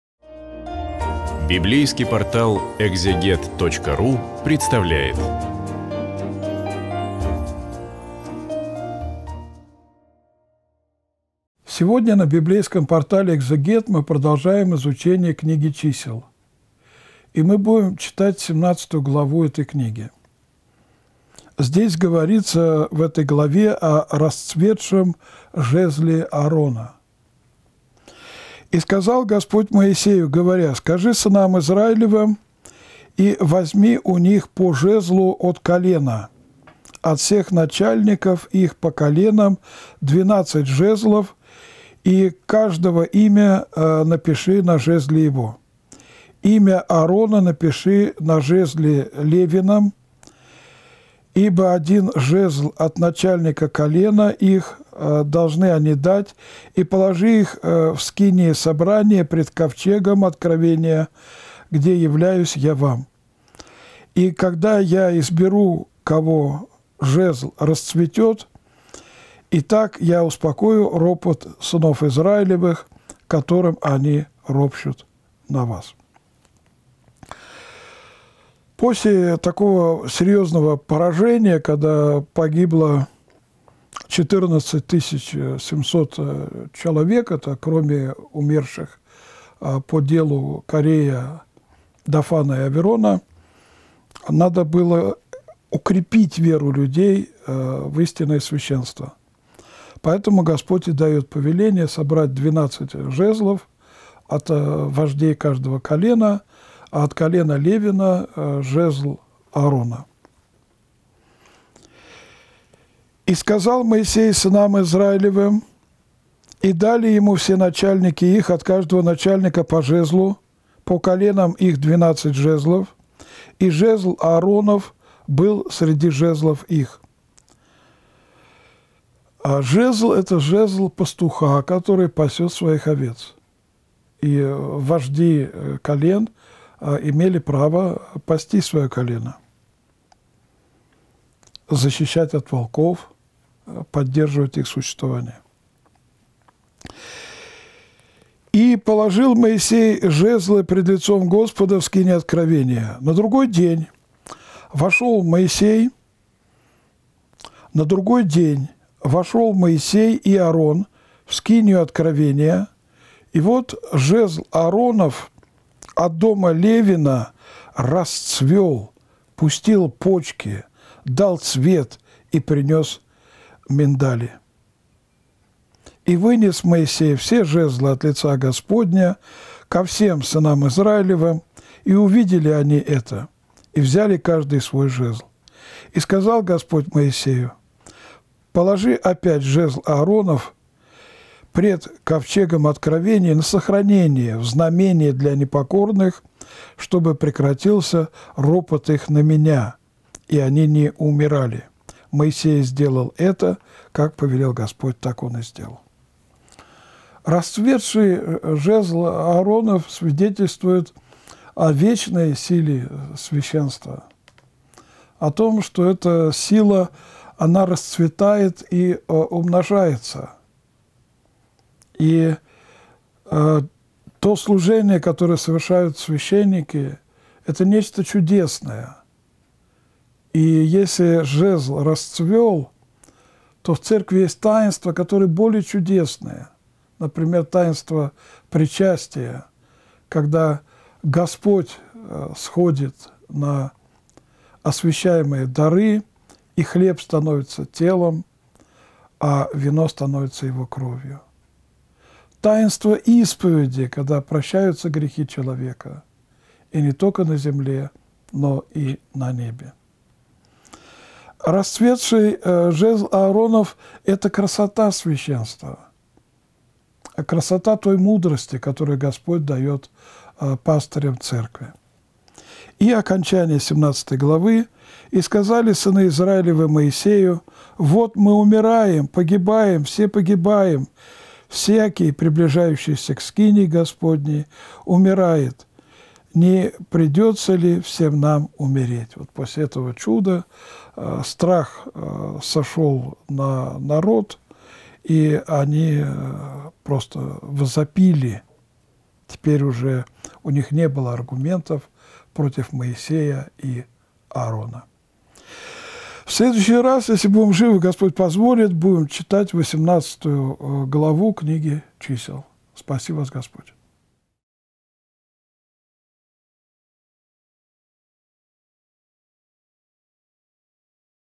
Числа. Глава 17. Протоиерей Олег Стеняев. Ветхий Завет
Продолжаем цикл лекций по книге Чисел. Разбираем 17-ую главу с протоиереем Олегом Стеняевым.
 Множество раз разными чудесами Бог показывал Свое благоволение к тем людям, которых Он избрал. Почему это чудо особенно? Зачем расцветший жезл Аарона помещать со скрижалями и манной в Ковчег? Тот сухой посох, который всего лишь за единственную ночь в пустыне пустил почки, дав цветы и плод миндаля, не является ли образом жестоковыйного Израиля, который в итоге смирился перед Богом и обрел неземную красоту Спасителя? Так и наша жизнь цветет и благоухает, если мы со Христом. Даже если мы очень сухи и жестоки, как засохший посох.